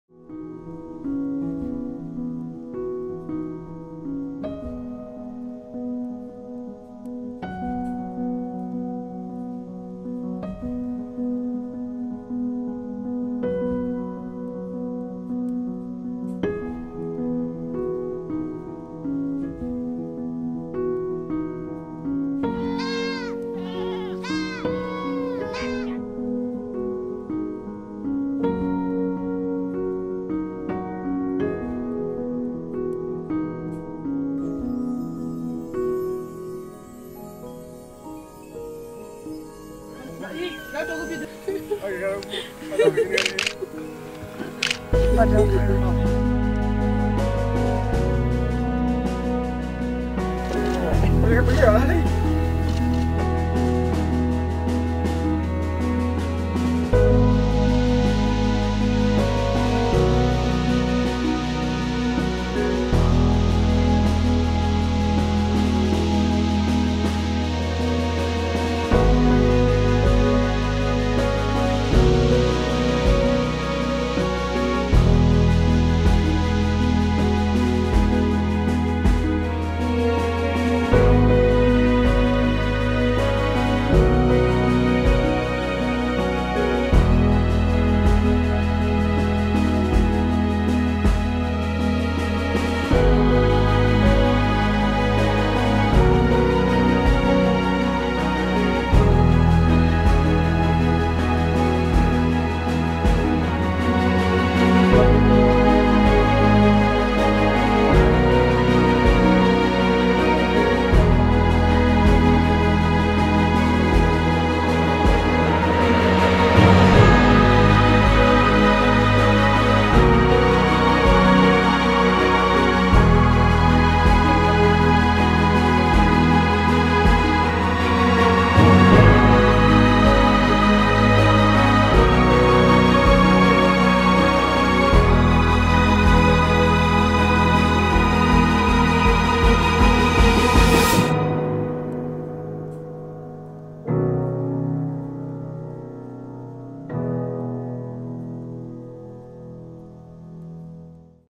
Behind the scenes - Himba Namibia - Jimmy Nelson
Take a look behind the scenes with Jimmy Nelson making a visual document of the magnificent Himba culture in Namibia.